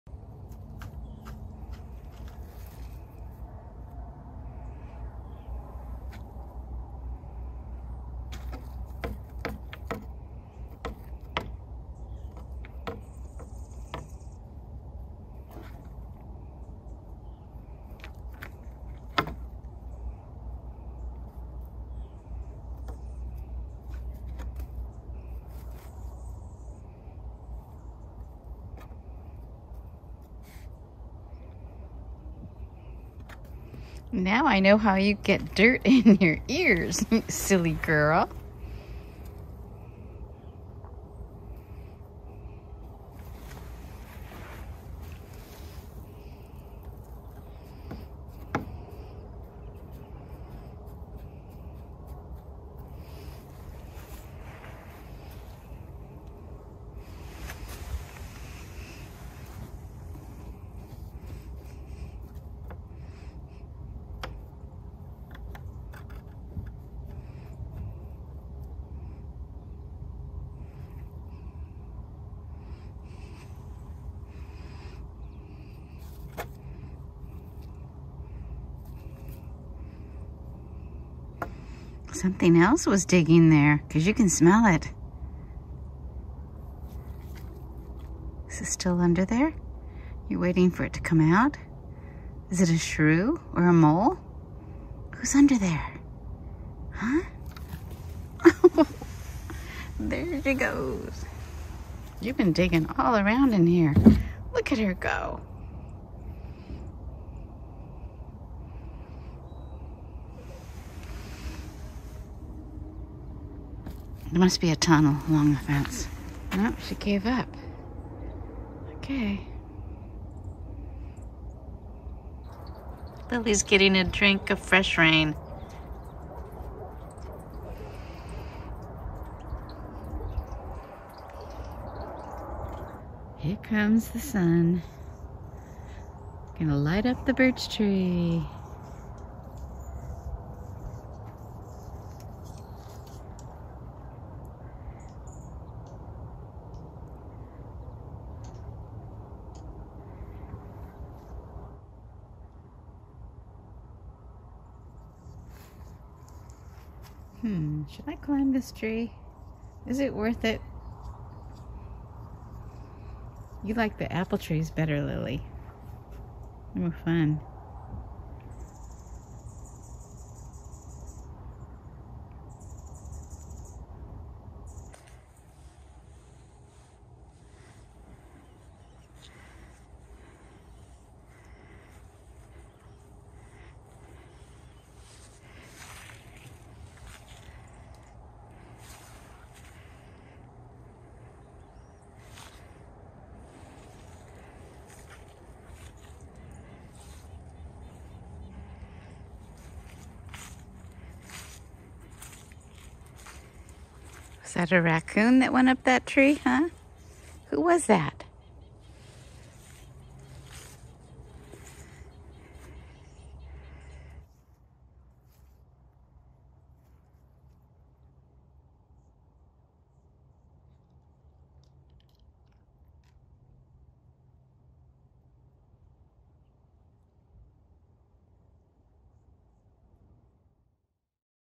Our Cat Digs Dirt Like a Dog!
Diane did a fantastic job discretely capturing Lily digging in the dirt along our fence line, more like a dog than a cat.

This is the first video recorded by Diane using her new iPhone 12 Pro. Beautiful cat and fall colors video Diane!

Lily, our Flame Point Siamese cat, now 2 and ½ years old, loves to explore our yard and gardens. She is especially adventurous when we make changes to our yard, her world.

I hope you enjoy watching this video montage and can relate to such funny cat anecdotes. Thank you for visiting our channel!

– Randall